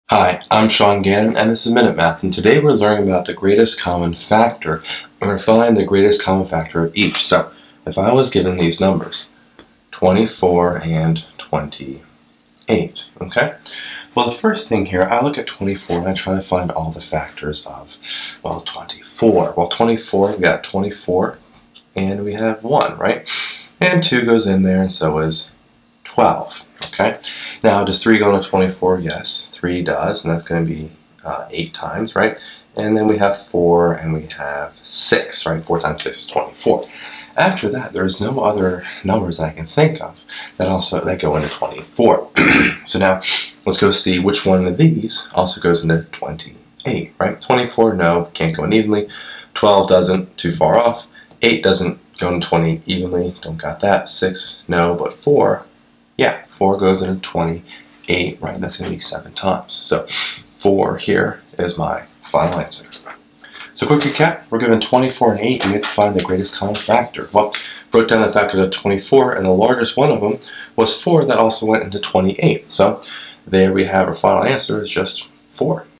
Find the Greatest Common Factor of 24 and 28
A math video lesson on the Greatest Common Factor (GCF) which is a topic in Number Theory in Pre-Algebra. This video discusses how to find the greatest common factor (GCF) of 24 and 28.